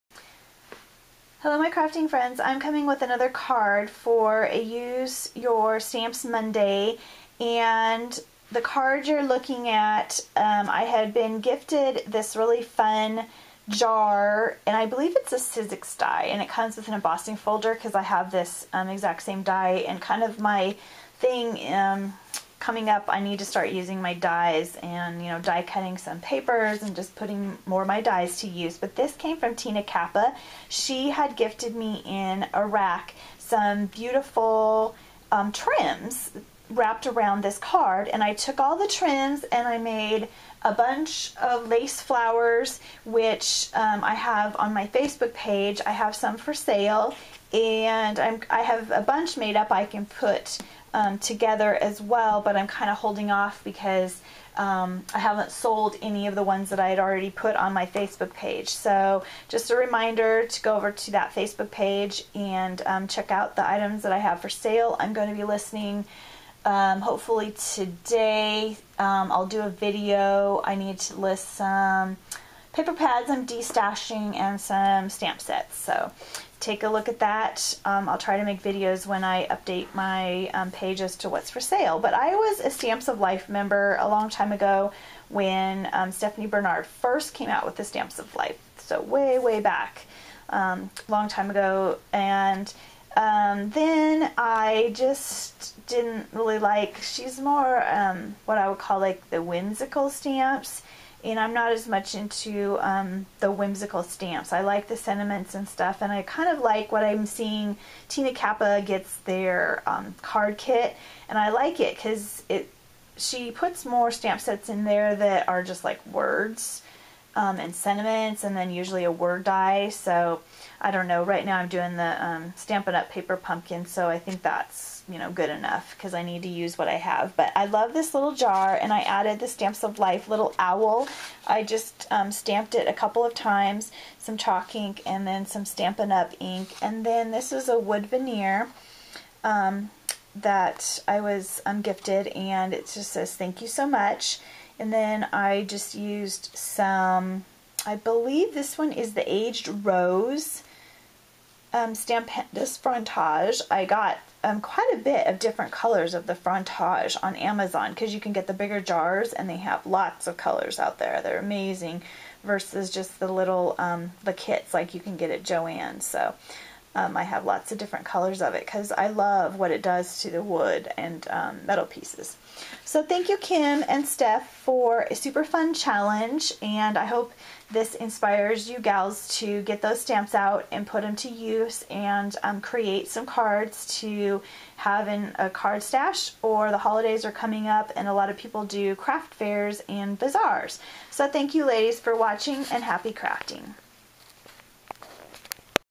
Use your stamps Monday #3
Another card I created using an old Stamps of Life stamp set.  TFW  Join in the challenge that brownbabycraftaholic and kim possible have each month to use our stamps.  TFW